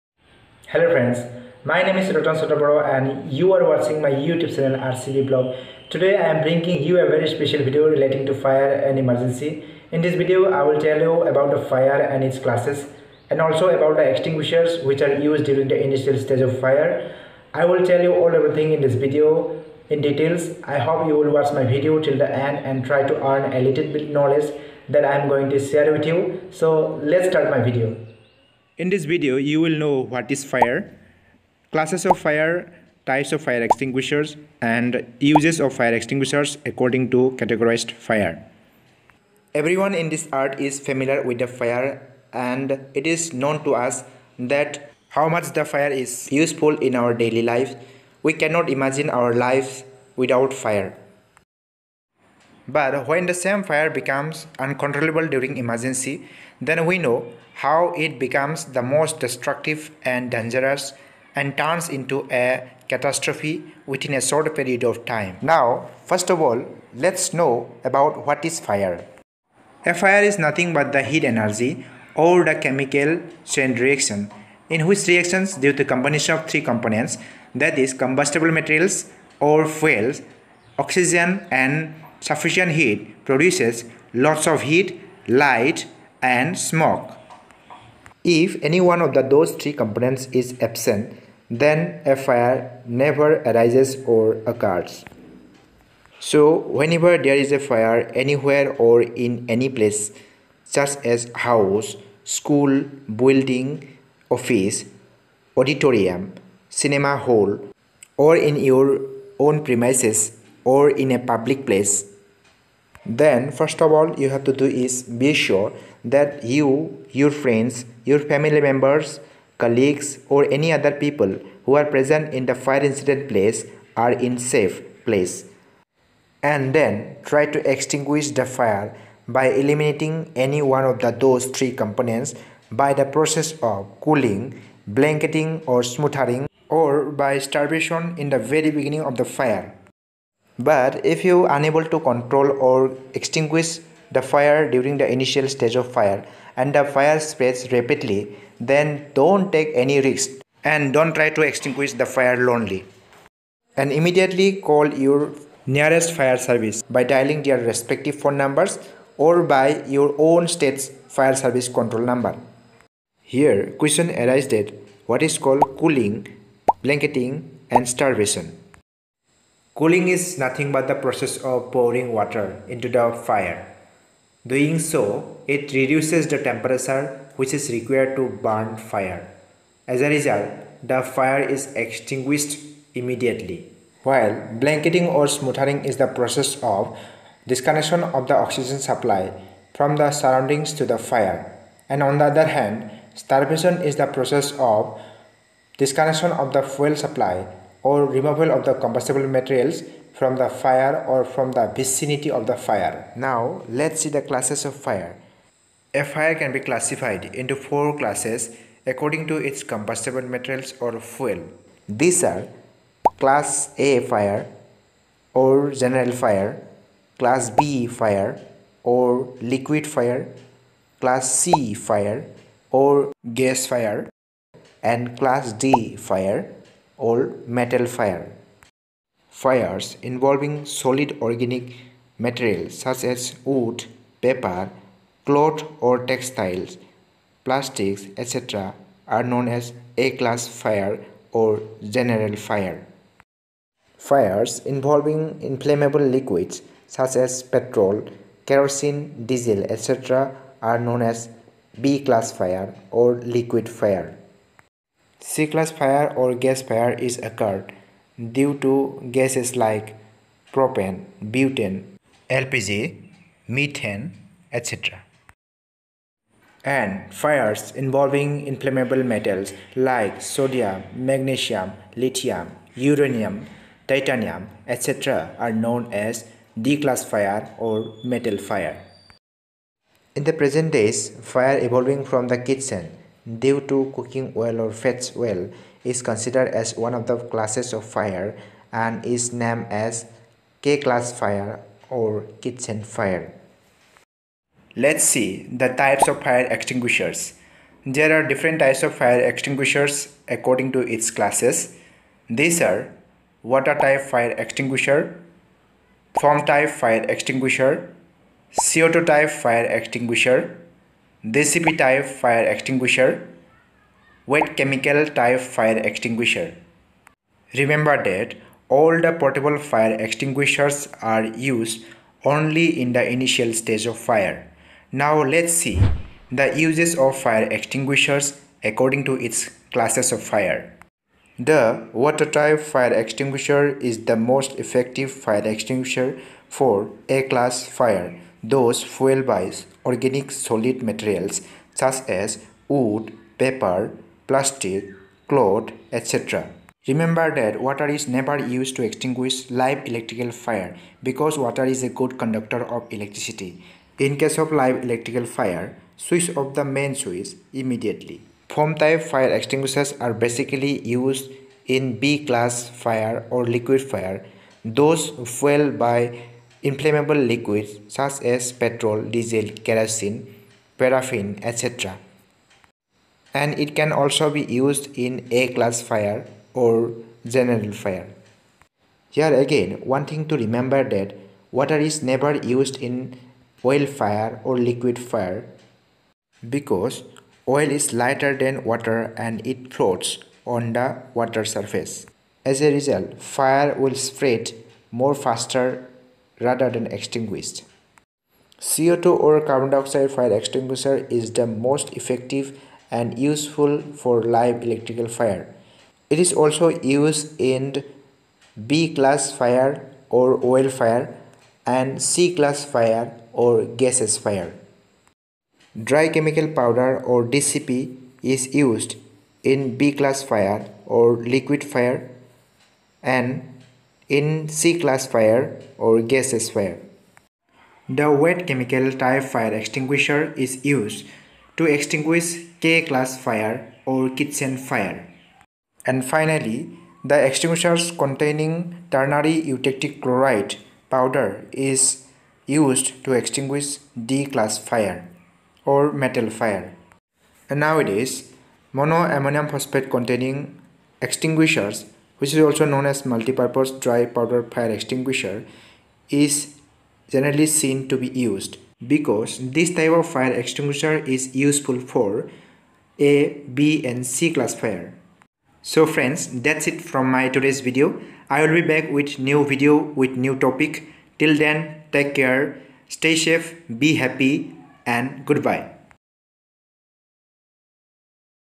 Fire & Emergency, What is a Fire?, Types of the Fire Extinguishers and their Uses
This video is related to the fire safety and preventive measures. In this video, the basic knowledge about the Fire, the classes of the fire, the types of Fire Extinguishers and their uses during the initial stage of fire are discussed in detail.